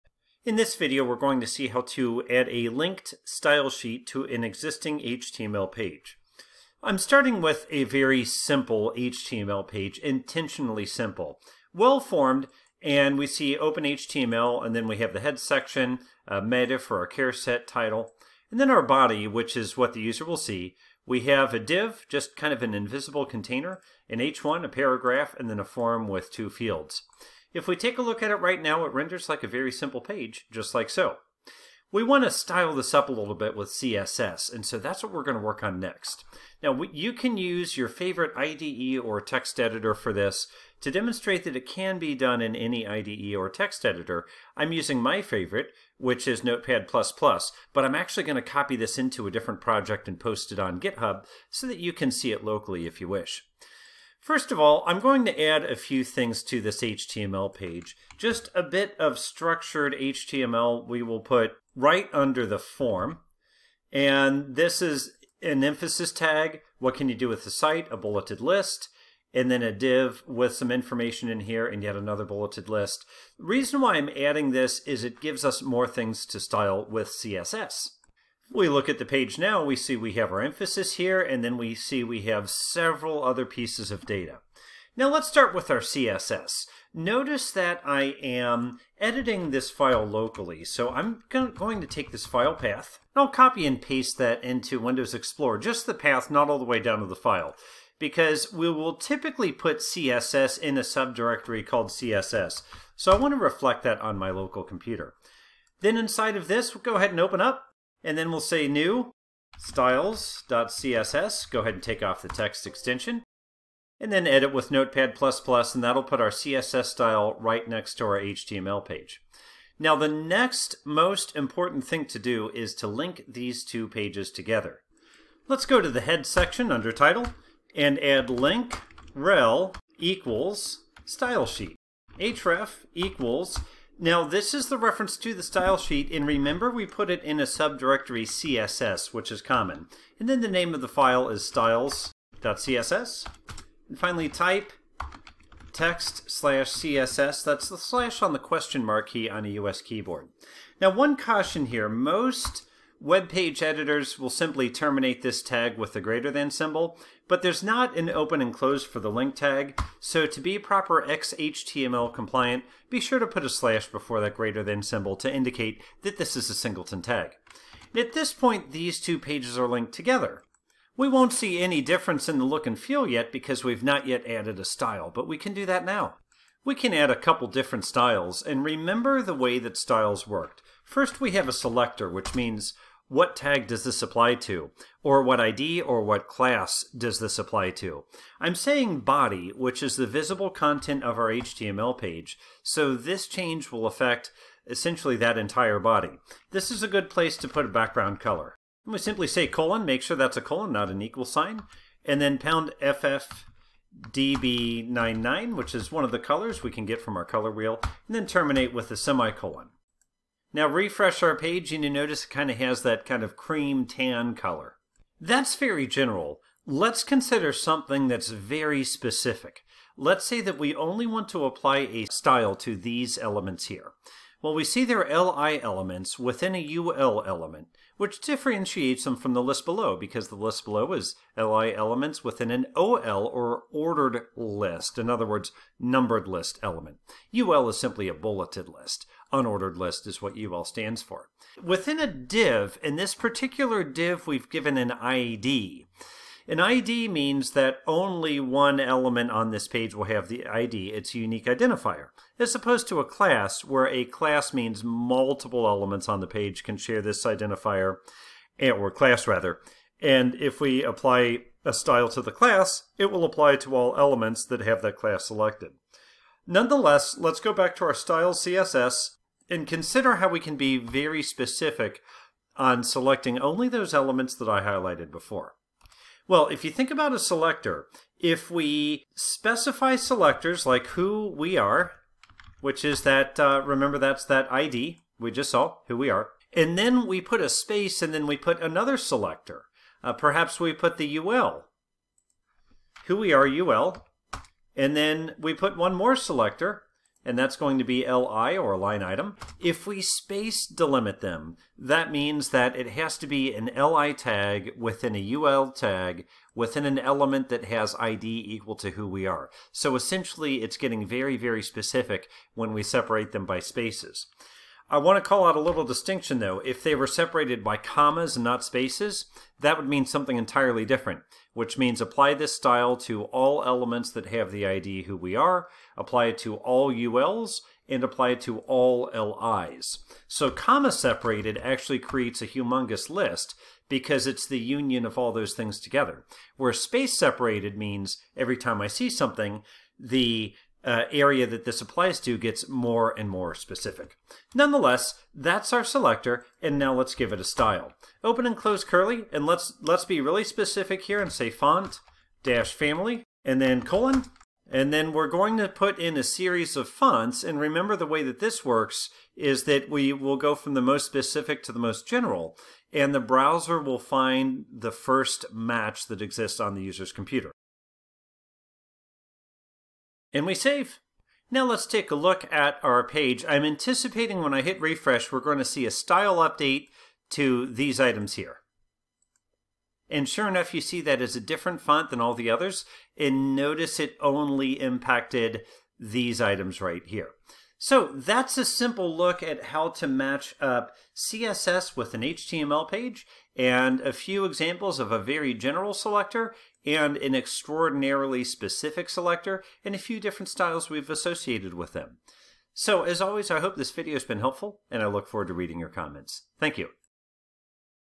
Link a CSS page to an HTML page, Create a few Styles
Start with a simple HTML page.  Add a linkned CSS stylesheet, and then show how to specify a general background color for the entire page, and show how to create a cascading set of elements, to style a very specific element.